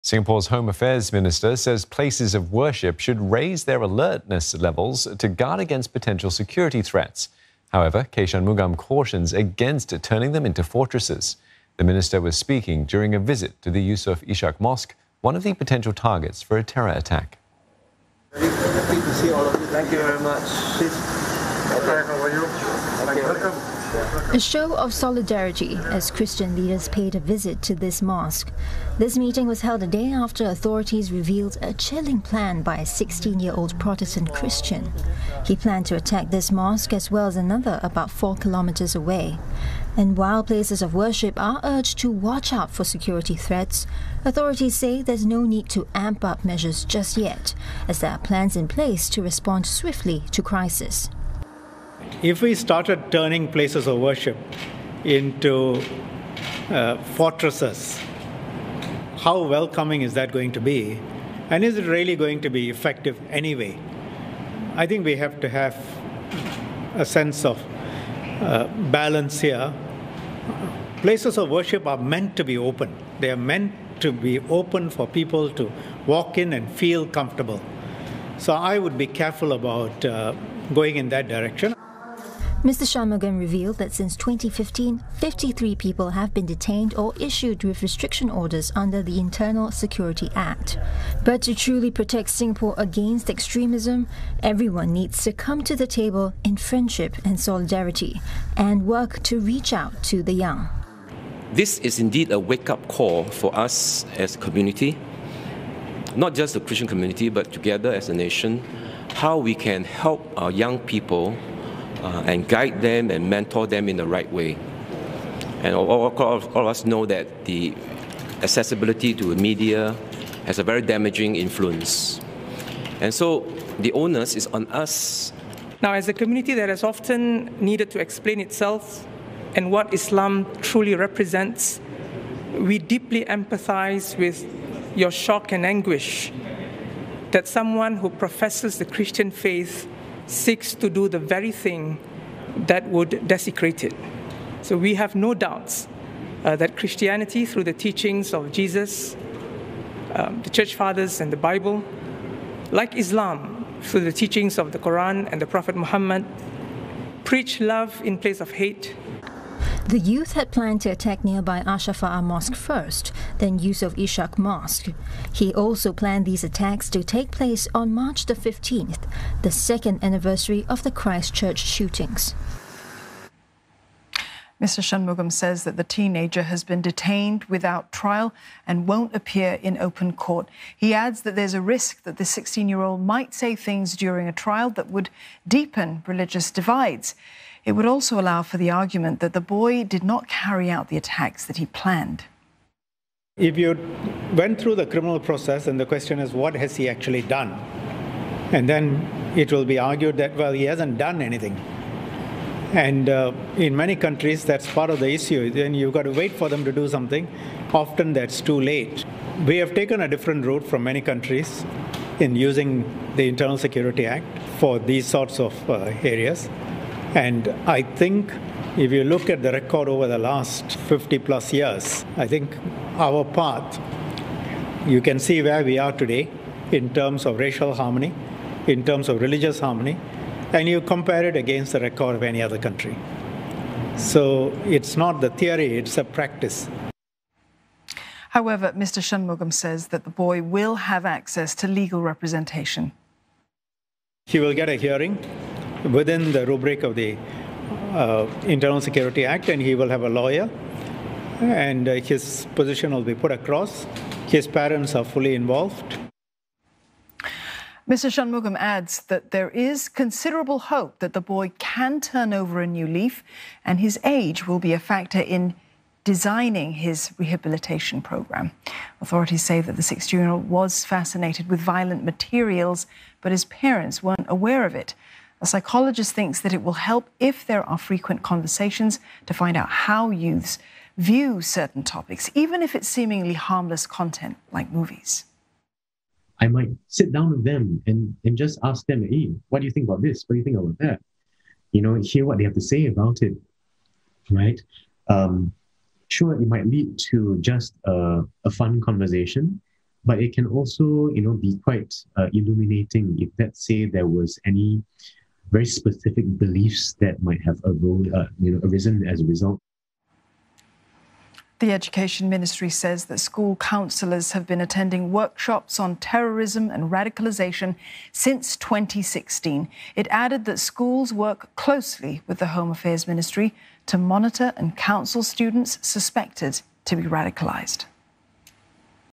Singapore places of worship should raise alertness levels: K Shanmugam
Singapore's Home Affairs Minister has said that places of worship should raise their alertness levels to guard against potential security threats. However, K Shanmugam cautioned against turning them into "fortresses". The minister was speaking during a visit to the Yusof Ishak Mosque, one of the potential targets for a terror attack.